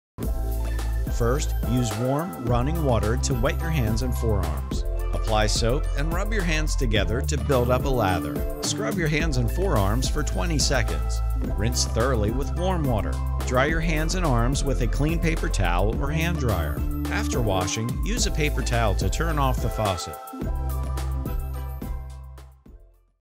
How to Wash Your Hands
Proper hand washing is vital for upholding food safety standards in commercial foodservice settings. To wash your hands properly, use warm running water to wet both your hands and forearms. Apply soap and then rub your hands together to build a lather for 20 seconds; make sure you also scrub your nails and fingertips to remove grime.

Train your staff in how to wash their hands correctly to prevent the spread of disease and reduce the risk of foodborne illnesses. Provide easy access to clean paper towels so they can dry their clean hands and forearms. They will also need a paper towel to turn off the faucet; otherwise, they will contaminate their clean hands.